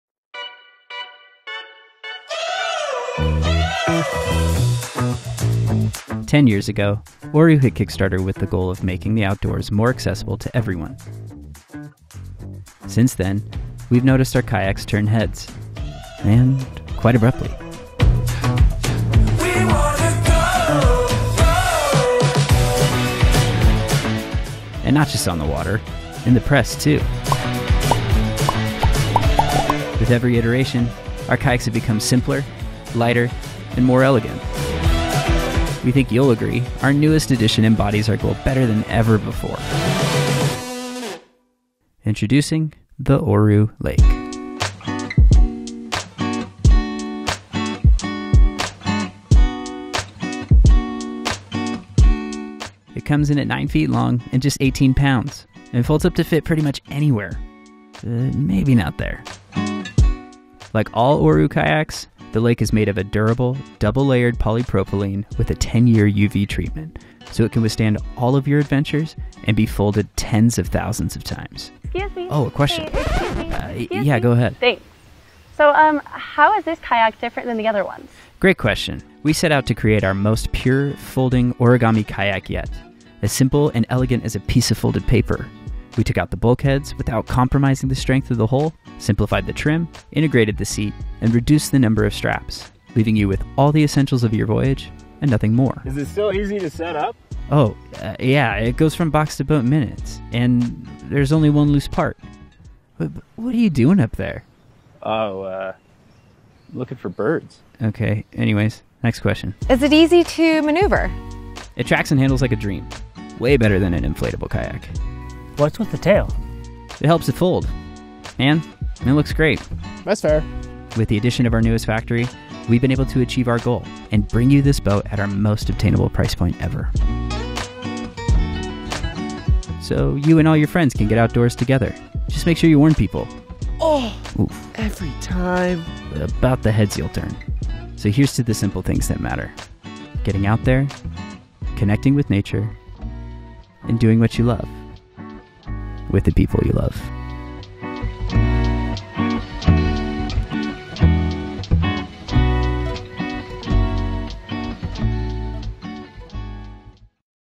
The Lightest, Simplest Origami Kayak Ever | Oru Lake Kayak
The Oru Lake Kayak is the result of a 10-year-long innovation process that provides revolutionary ease to your outdoor paddling adventures. For over a decade developers have been carefully designing and testing a kayak that is developed utilizing origami technology. The long design process has finally resulted in the Oru Lake Kayak - the simplest, lightest origami kayak in the game.

You have never seen a kayak so easily usable before. The origami design allows for the entire product to be put together in minutes - tests have shown that the Oru Lake Kayak only takes 1-2 minutes to assemble! The material used in the production of the kayak is called OruPlast, a custom extruded, UV-treated plastic. Each kayak is made of one seamless sheet of double-walled plastic, ensuring that the kayak is durable and the material stays tough even through countless folding.

The Oru Lake Kayak folds into a tight package that you can easily carry with you. It fits into the back of your car effortlessly, eliminating the need for a roof rack when you embark on your kayaking trip! If you want to paddle in international waters, you can also simply check the folded kayak on a plane. Furthermore, you don’t need to worry about the kayak being heavy! With a weight of a mere 18 lbs (8 kg), the Oru Lake Kayak is so light even a child can carry it. The kayak itself can carry up to 250 lbs (113 kg) in weight.

Get ready for a revolutionary kayaking experience with this simple, foldable product. The transparent material of the Oru Lake Kayak allows for you to be closer to the water than ever before as you are paddling seated in the open, extremely stable cockpit. You simply need to get your folded kayak, get close to water, unfold in only 1-2 minutes, and the Oru Lake Kayak has you on a paddling adventure in close to no time!